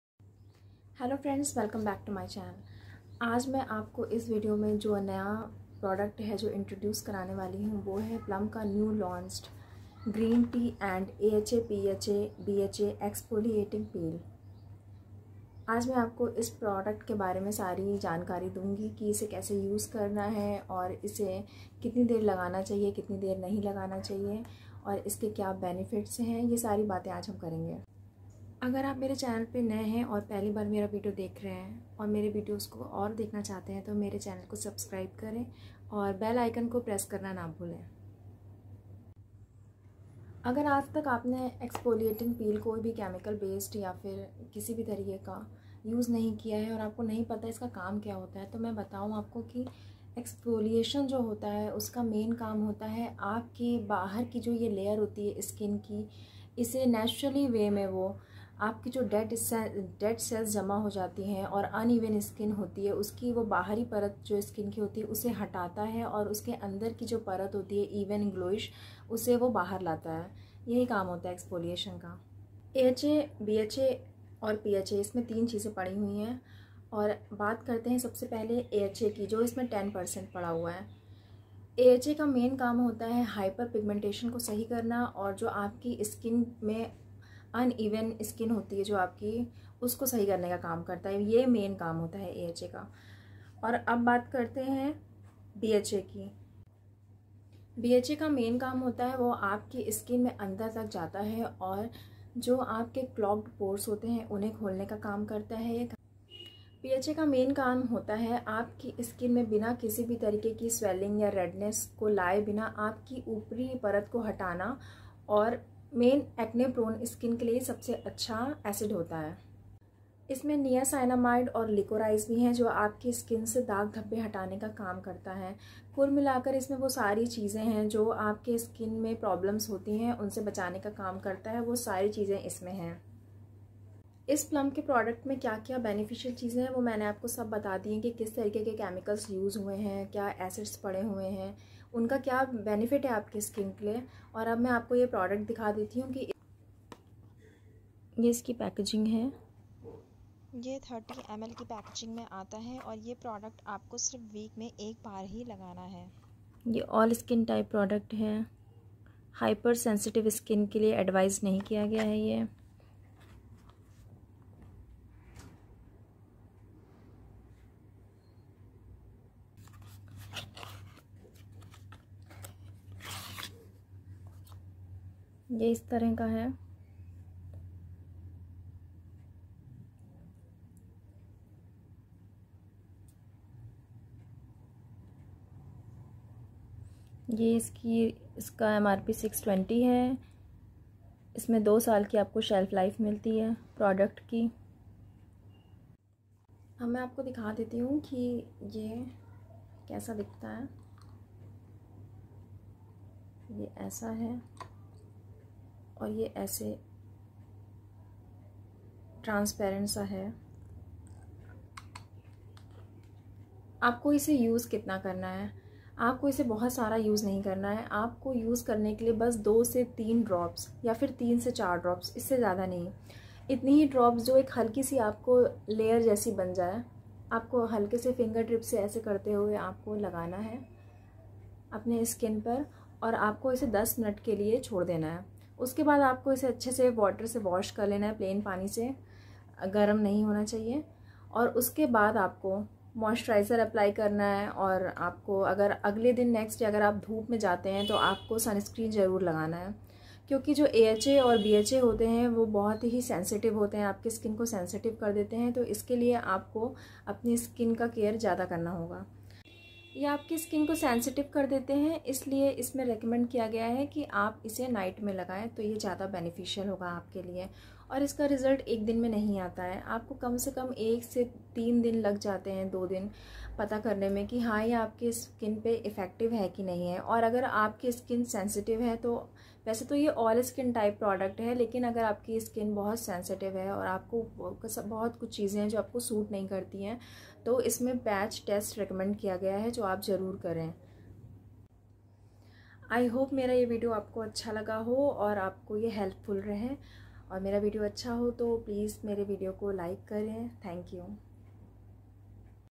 Plum Green Tea & AHA,PHA,BHA Exfoliating Peel Review/What is AHA, BHA, PHA/why are they required??
Hello friends,

Today's video is about New launched plum exfoliating peel

I shared my view about the product.

I hope you find it helpful.

Shruti💞